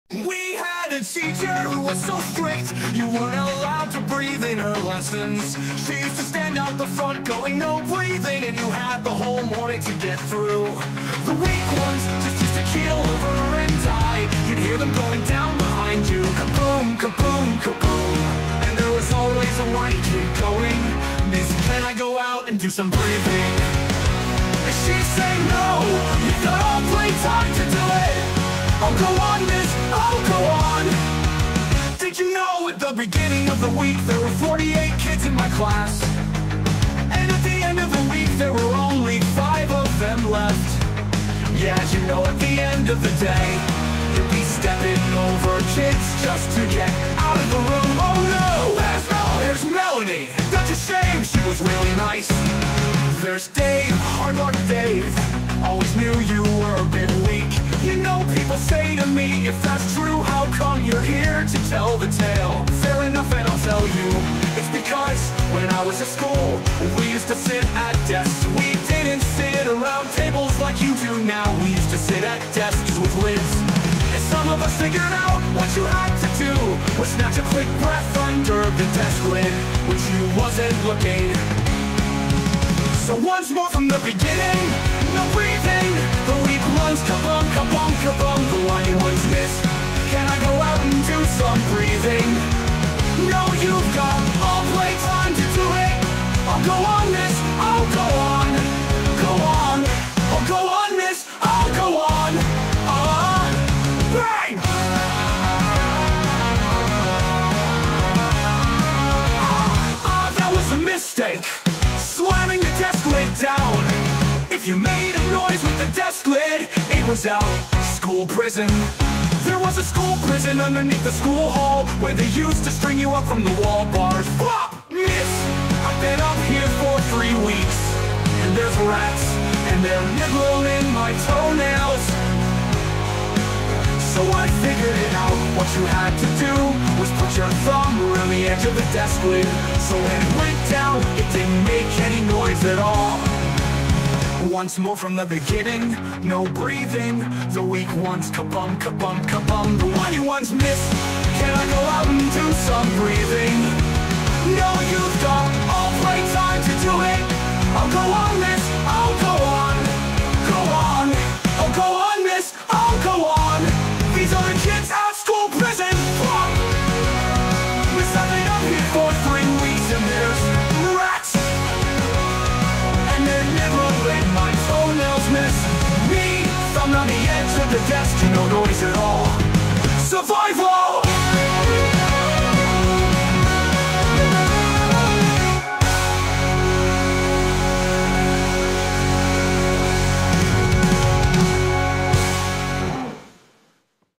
Plum 41 - No Breathing
We had a teacher who was so strict,
you weren't allowed to breathe in her lessons.
She used to stand out the front going,
"No breathing!"
And you had the whole morning to get through.

The weak ones just used to keel over and die,
you'd hear them going down behind you.
Ka-boom! Ka-boom! Ka-boom!
And there was always a whiny kid going,
"Miss, can I go out and do some breathing?"
and she'd say, "No, you've got all playtime to do it!"
"Oh go on Miss, oh go on!"

Did you know at the beginning of the week
there were forty eight kids in my class,
and at the end of the week there were only five
of them left.
Yeah, d'you know at the end of the day
you'd be stepping over kids just to get out of the room.

Oh no!
There's Melanie!
That's a shame, she was really nice!

There's Dave.
Hard luck Dave,
always knew you were a bit weak.

You know people say to me
"If that's true, how come you're
here to tell the tale?"
Fair enough and I'll tell you.
It's because, when I was at school,
we used to sit at desks. We didn't sit around
tables like you do now, we used to sit at desks,
with lids.

And some of us figured out, what you had to do
was snatch a quick breath under the desk lid
when she wasn't looking.

So once more from the beginning.

"No breathing!"
The weak ones, Ka-bum, ka-bum, ka-bum.
The whiny ones, "Miss, can I go out and do some breathing?"
"No, you've got all playtime to do it!"

Bang!

Ahh! That was the mistake!
Slamming the desk lid down!
If you made a noise with the desk lid it was-
"Out! School Prison!"
There was a school prison underneath the school hall where they used to string you up from the wall bars.

FWOP!
"Miss, I've been up here for 3 weeks!
And there's rats and they're nibblin' my toenails!"

So I figured it out, what you had to do
was put your thumb 'round the edge of the desk lid,
So when it went down, it didn't make any noise at all.

Once more, from the beginning.

"No breathing..."
The whiny ones! "Miss,
can I go out and do some breathing?"

These other kids, "Out! School Prison!"
FWOP!
and there's rats!
And they're nibbling my toenails, Miss!"

Me , thumb 'round the edge of the desk.
No noise at all.

Survival!